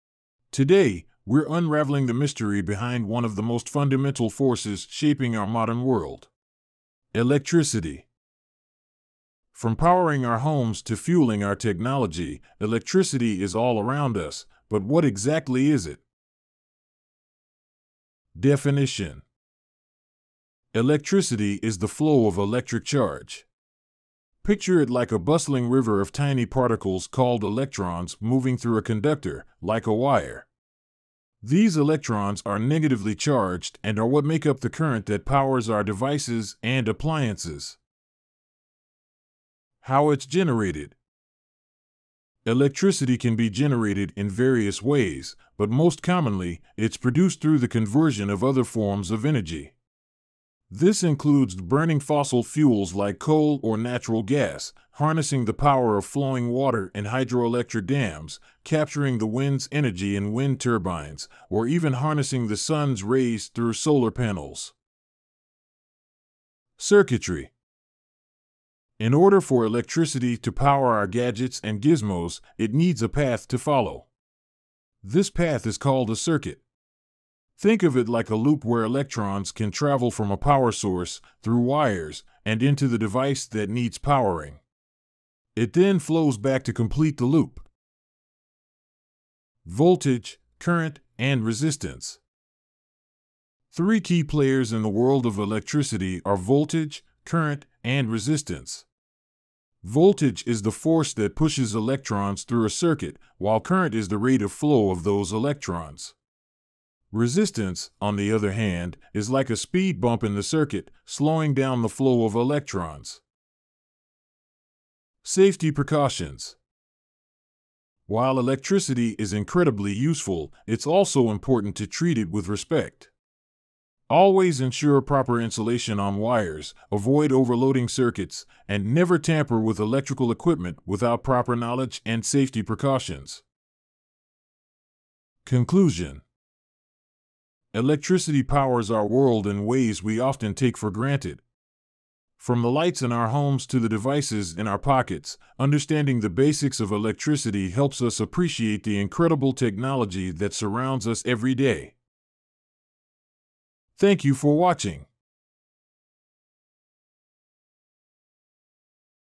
Maybe We Use It Every Day But How Well Do We Know It? (Electricity)
Electricity is the flow of electric charge, carried by tiny particles called electrons. It's generated through various methods like burning fossil fuels, harnessing water or wind energy, or using solar power. Electricity flows through circuits, propelled by voltage, and encountering resistance along the way. Safety precautions are crucial when dealing with electricity. Understanding its basics helps us appreciate its role.

Definition:
Electricity is the flow of electric charge. Picture it like a bustling river of tiny particles called electrons moving through a conductor, like a wire. These electrons are negatively charged and are what make up the current that powers our devices and appliances.
How It's Generated:
Electricity can be generated in various ways, but most commonly, it's produced through the conversion of other forms of energy. This includes burning fossil fuels like coal or natural gas, harnessing the power of flowing water in hydroelectric dams, capturing the wind's energy in wind turbines, or even harnessing the sun's rays through solar panels.
Circuitry:
In order for electricity to power our gadgets and gizmos, it needs a path to follow. This path is called a circuit. Think of it like a loop where electrons can travel from a power source, through wires, and into the device that needs powering. It then flows back to complete the loop.
Voltage, Current, and Resistance:
Three key players in the world of electricity are voltage, current, and resistance. Voltage is the force that pushes electrons through a circuit, while current is the rate of flow of those electrons. Resistance, on the other hand, is like a speed bump in the circuit, slowing down the flow of electrons.
Safety Precautions:
While electricity is incredibly useful, it's also important to treat it with respect. Always ensure proper insulation on wires, avoid overloading circuits, and never tamper with electrical equipment without proper knowledge and safety precautions.